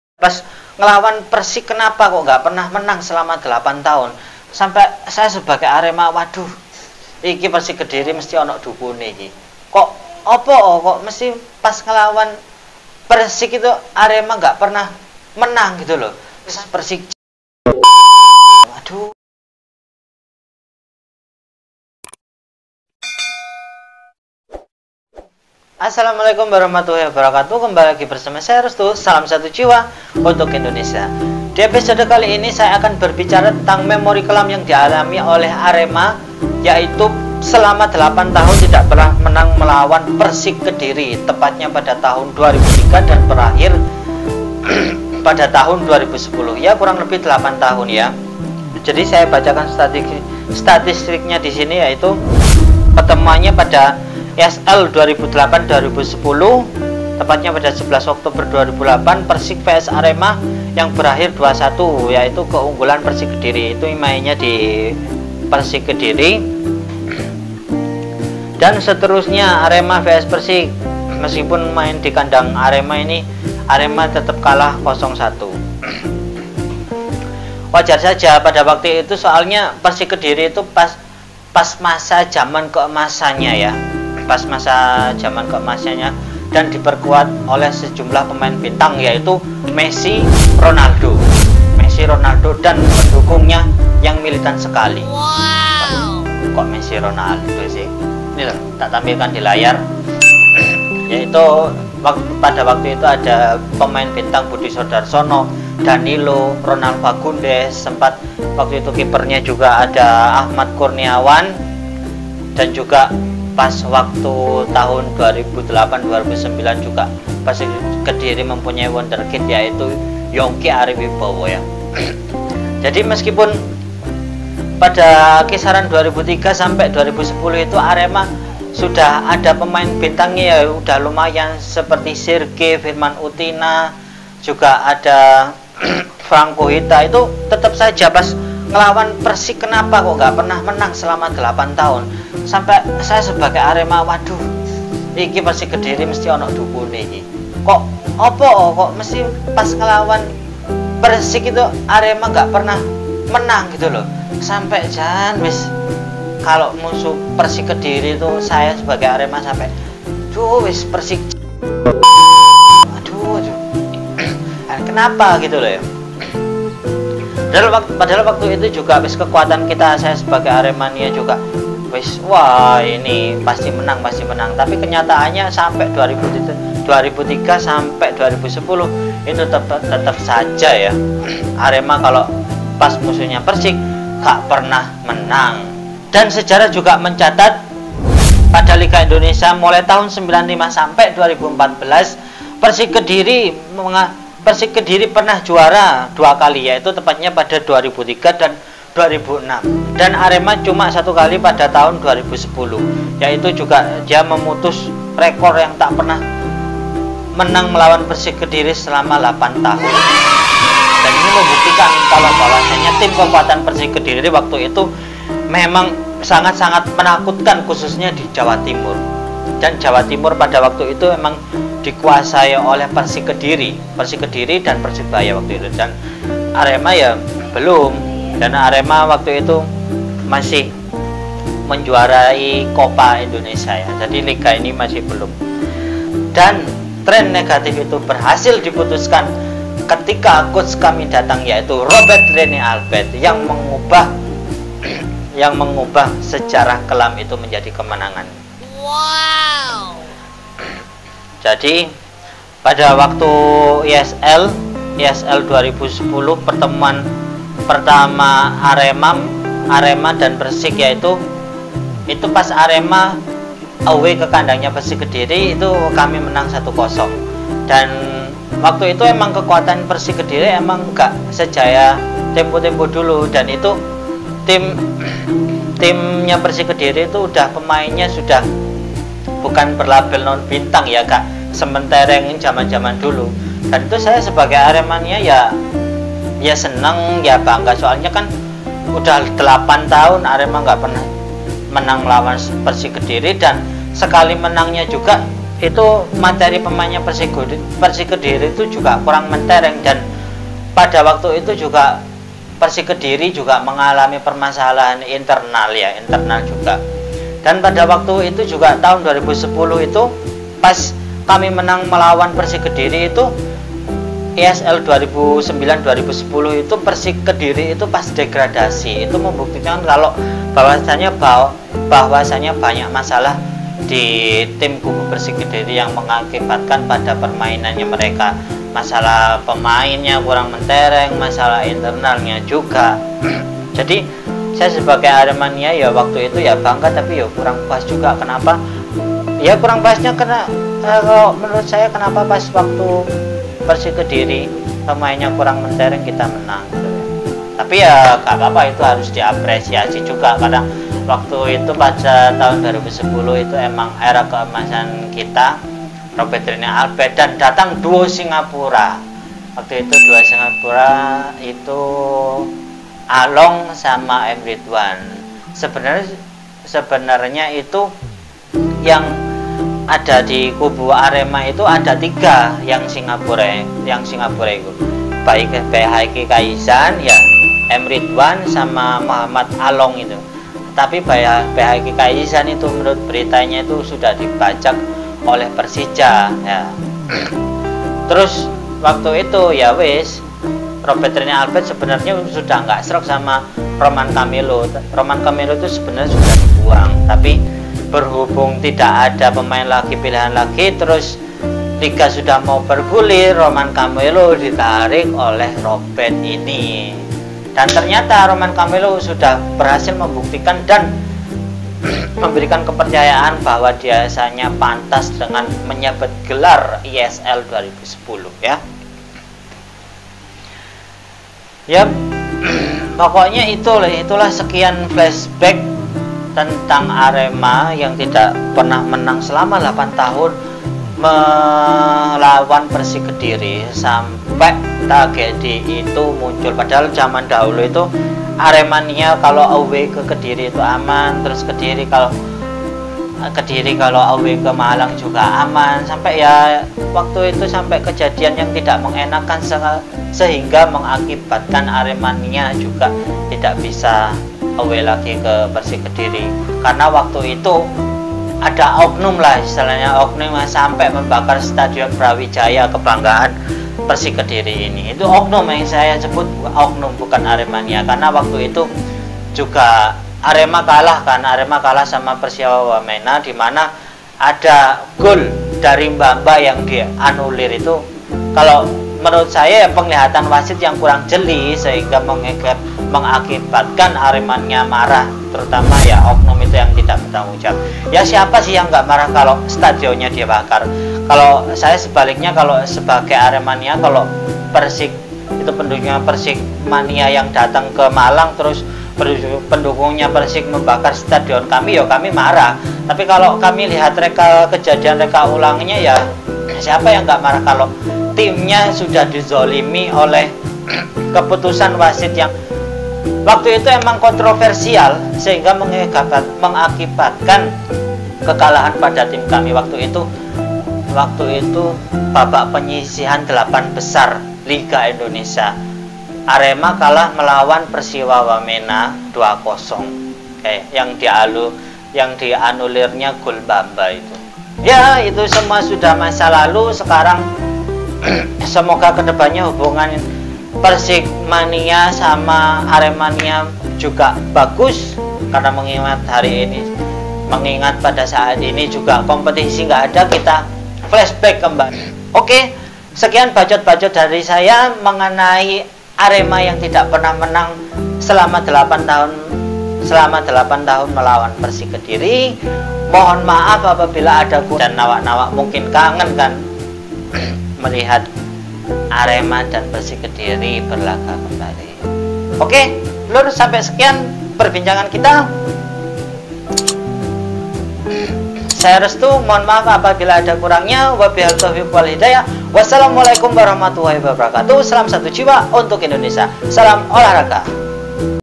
8 Tahun Arema Tidak Menang Melawan Persik
8 Tahun Arema Tidak Menang Melawan Persik

di episode kali saya saya bercerita atau flasback tentang sejarah pertemuan antara arema melawan persik kediri yang tidak pernah menang selama 8 tahun mulai tahun 2013 sampai berakhir 2010 ,
hal ini sungguh janggal karena sejatinya kekuatan tim waktu itu merata, serta waktu itu persik kediri diperkuat oleh pemain bintangnya yaitu Cristian gonzales , ronal fagundes, dan danilo serta banyak lagi yang lain yuk tonton aja jangan lupa like koment dan share yah terimakasih